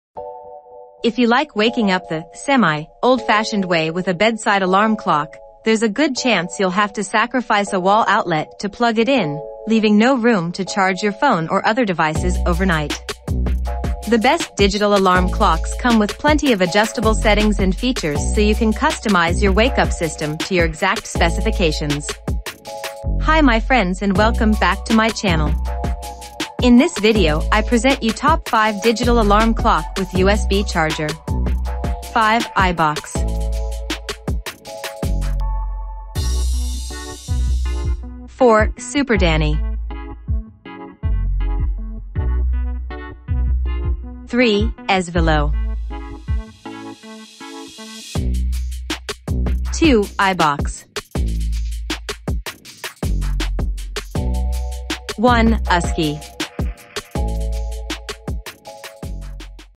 TOP 5 Digital Alarm Clock with USB Charger 2022
If you like waking up the (semi) old-fashioned way with a bedside alarm clock, there’s a good chance you’ll have to sacrifice a wall outlet to plug it in, leaving no room to charge your phone or other devices overnight. The best digital alarm clocks come with plenty of adjustable settings and features so you can customize your wakeup system to your exact specifications.